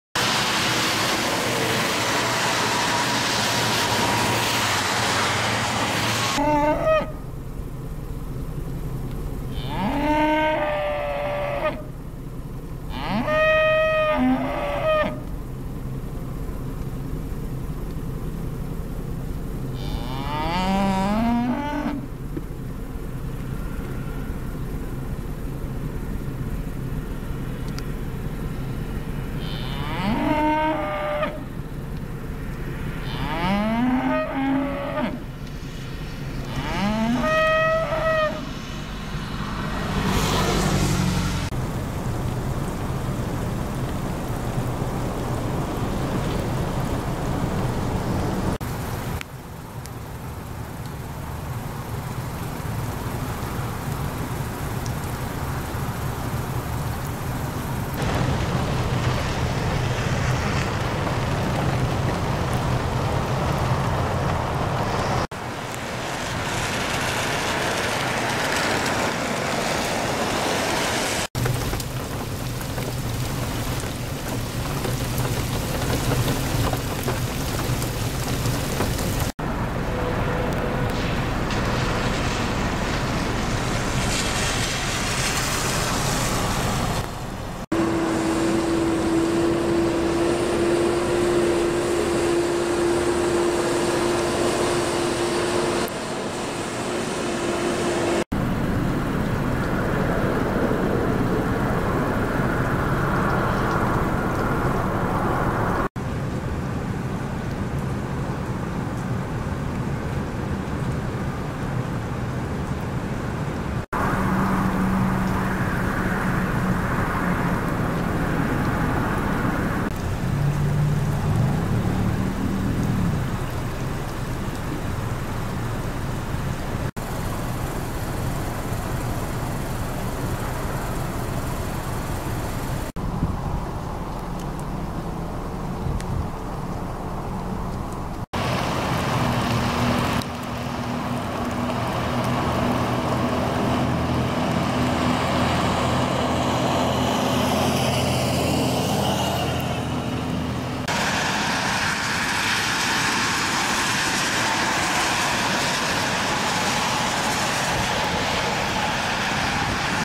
02-21-2018 Sanger, TX - Ice, Flooding, Cows Yelling, Branches Down
Clips from Sanger and Valley View, TX north of Denton of Wednesday's ice storm. Cows yelling in a field of icy grass, flooding on roads from two days of rainfall, icy pasture, bulldozer in Valley View gearing up to clean branches off roads, and multiple branches broken of various sizes from the freezing rain.